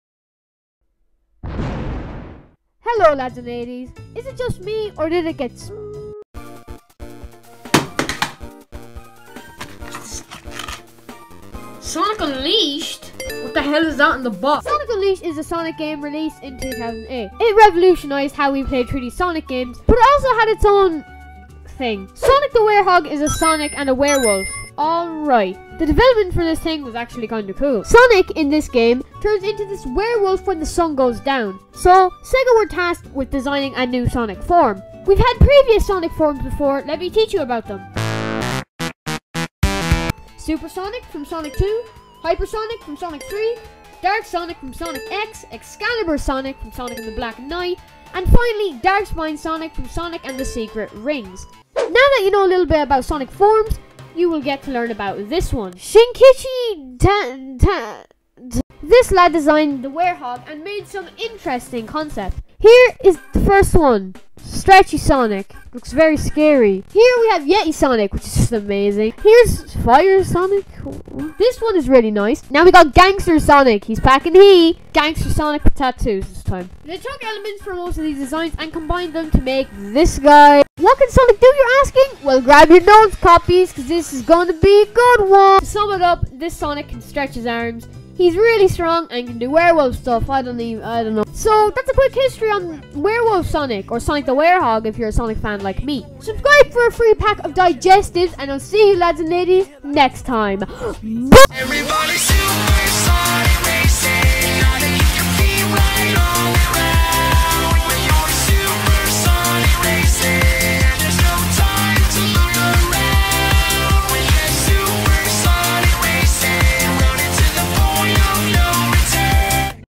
SonicTober 1: Sonic The Werehog
Today I’m gonna be looking at the concept and creation of Sonic the Werehog!